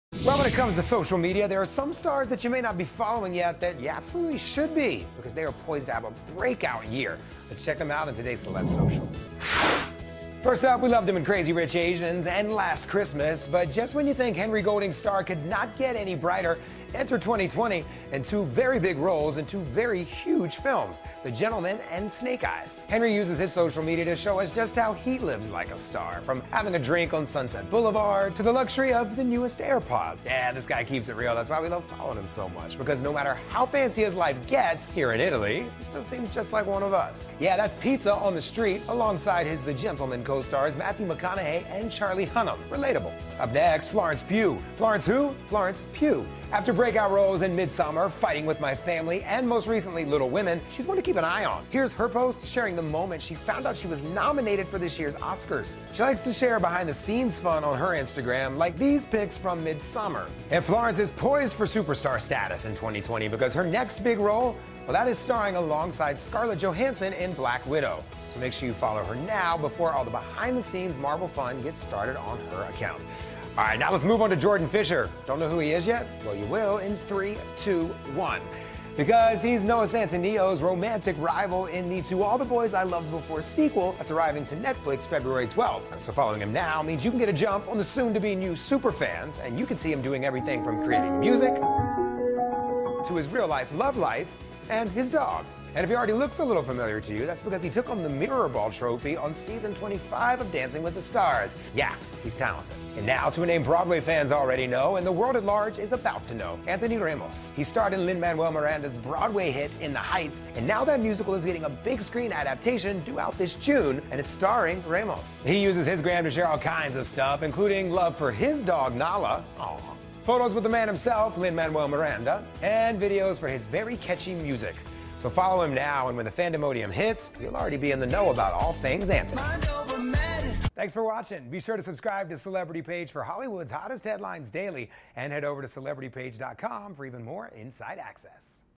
These Celebs Are MUST Follows On Social Media For 2020 | Celebrity Page
Jaymes is predicting BIG things for HenryGolding, Florence Pugh, Jordan Fisher, and Anthony Ramos. So naturally, we're taking a deep dive into their social media accounts to see what we can expect from these stars in 2020.